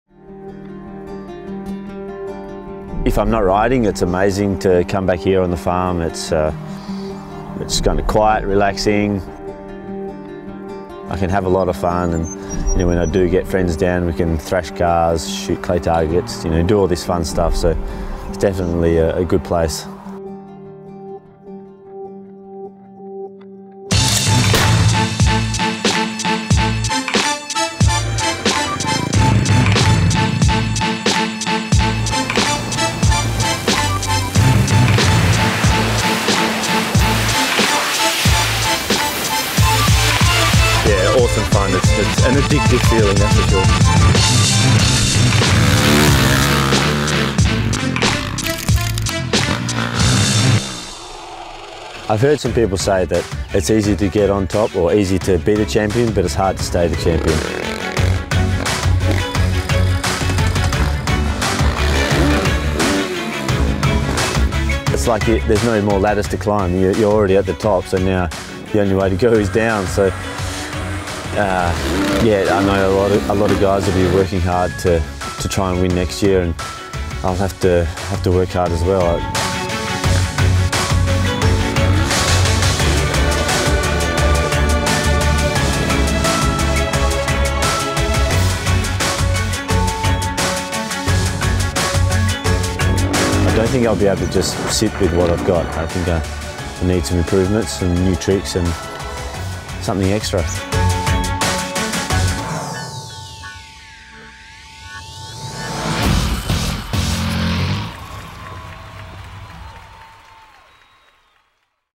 Josh Sheehan's Backyard Freestyle Motocross Playground
The Red Bull X-Fighters World Tour 2015 is about to start, and the 2014 Champion, Josh Sheehan, is back home in Australia getting all his tricks dialed for the new season. Take an inside look at Sheehan's backyard FMX dream setup, as he shoots shotguns, jumps out of airplanes, and of course, rides his dirtbike in preparation for Red Bull X-Fighters.